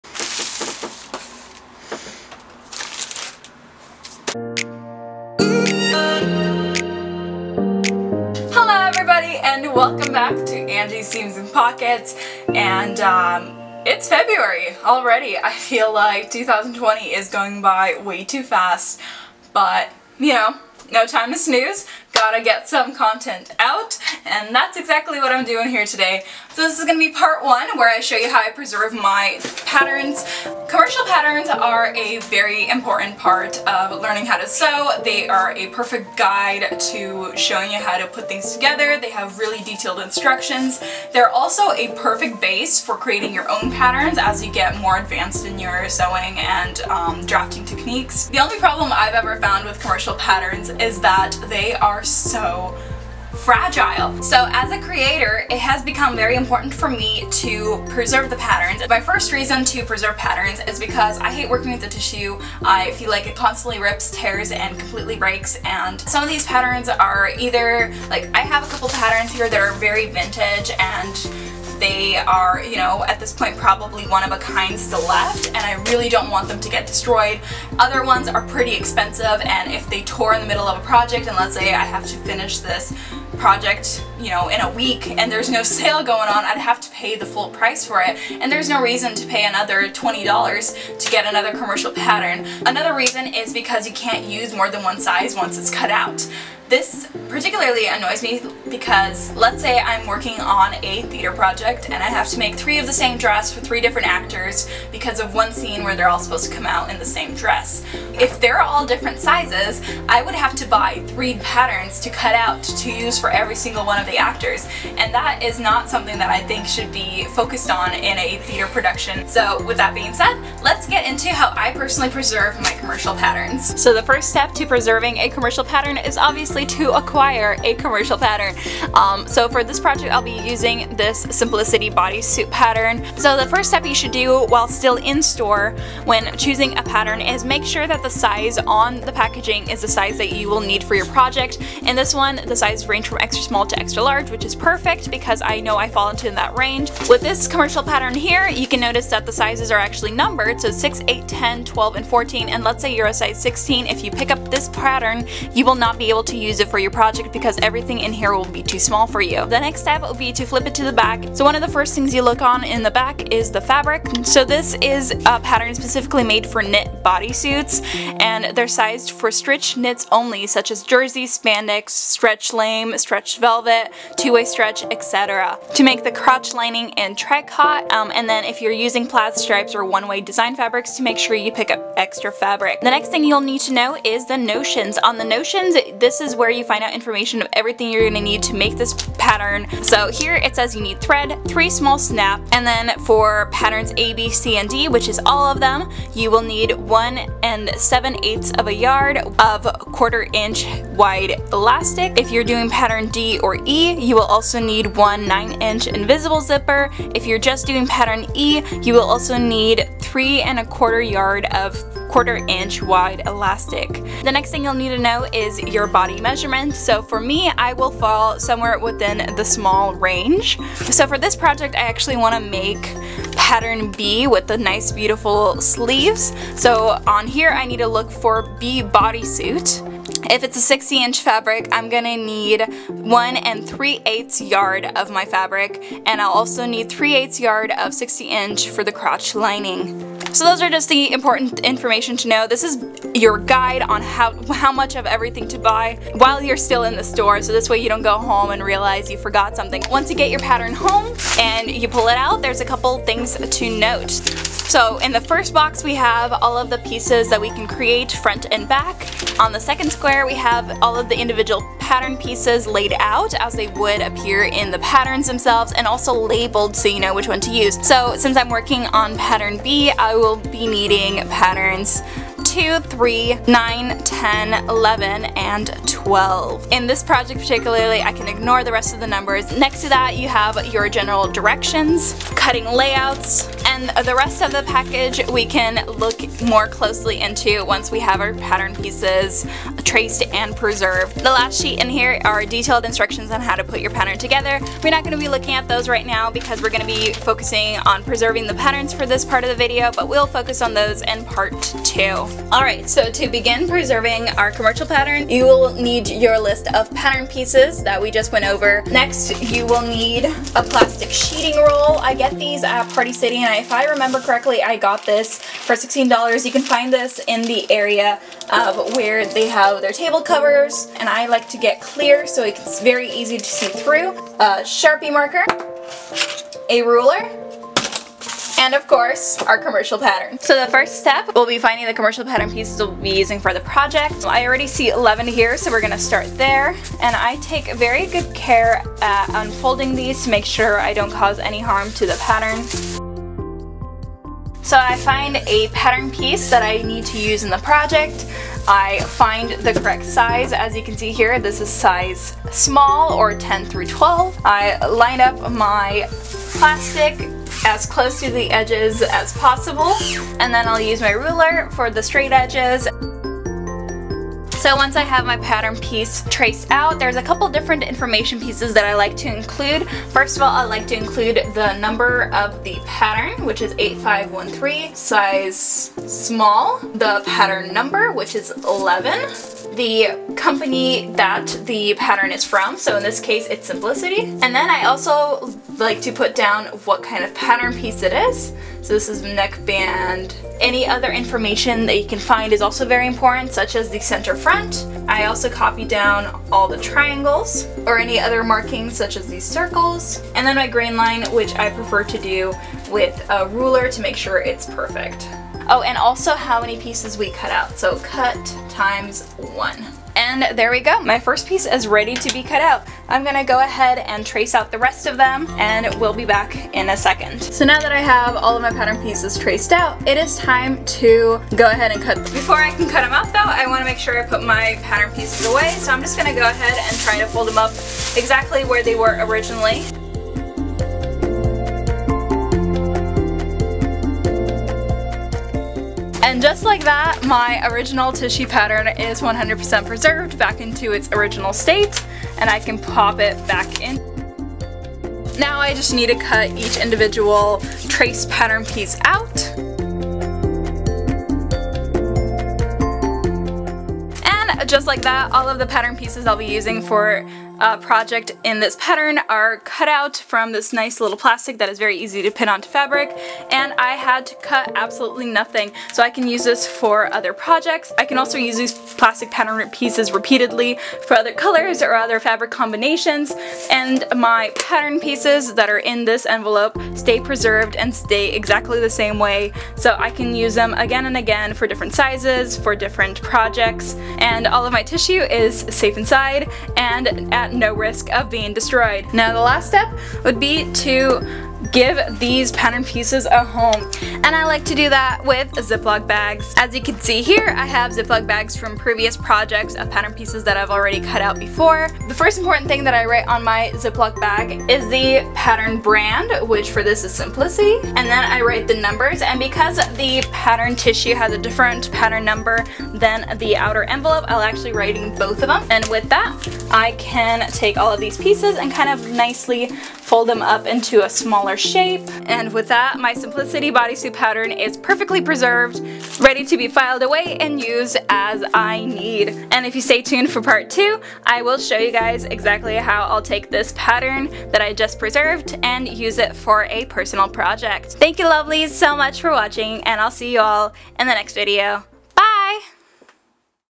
Commercial Patterns Pt. 1: Preserving the Pattern
This is going to be a 2 (maybe even 3) part series where I go over everything I know about using Commercial Patterns starting with preserving the original tissue.